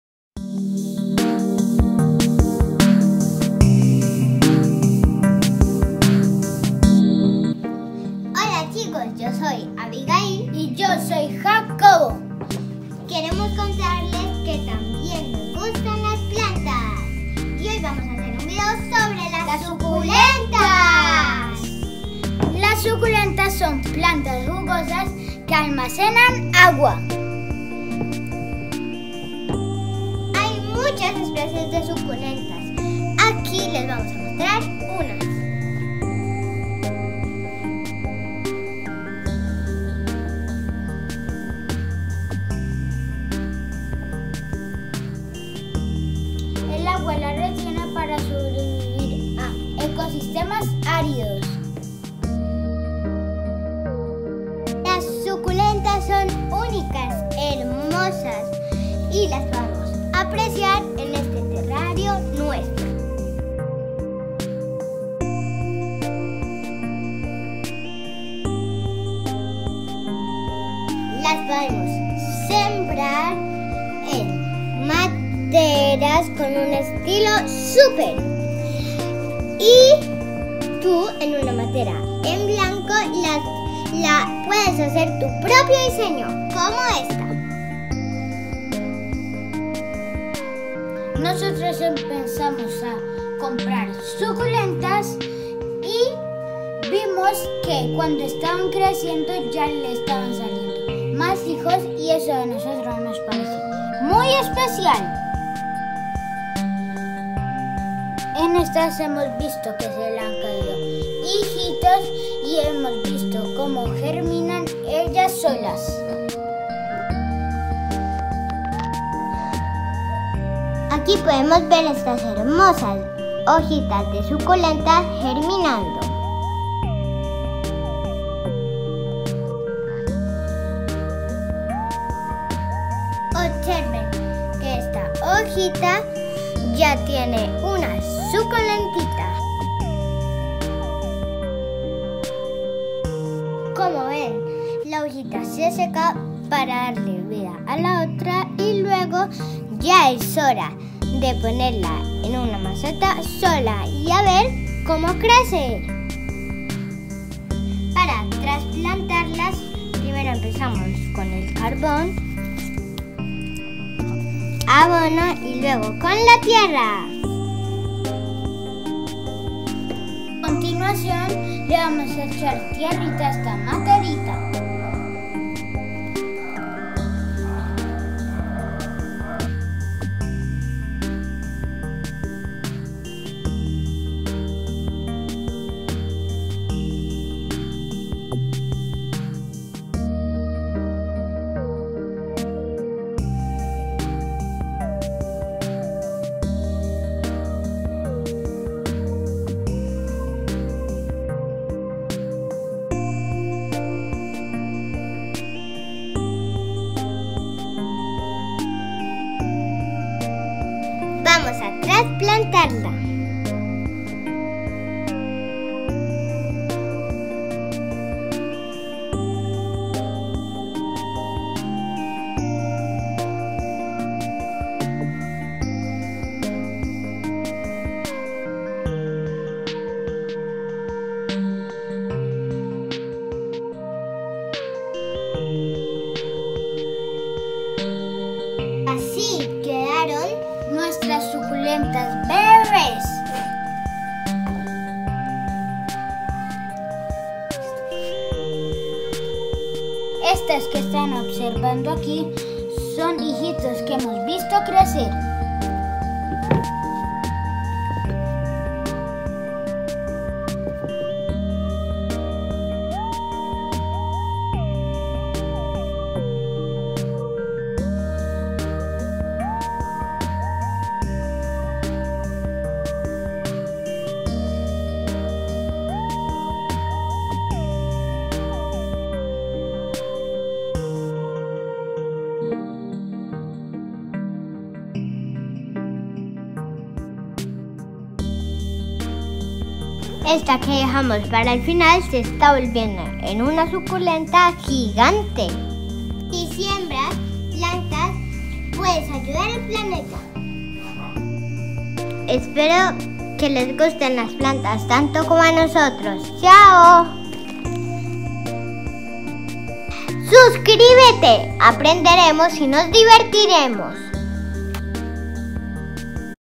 COMO TRANSPLANTAR SUCULENTAS BEBES
Las suculentas son un grupo de plantas que almacenan el agua en sus hojas, tallos y raíces. Las hojas tienden a ser gruesas y carnosas, pero algunos géneros y especies tienen hojas más gruesas que otras.

En este video te mostramos como se transplanta una suculentita y te mostramos algunas variedades de las suculentas.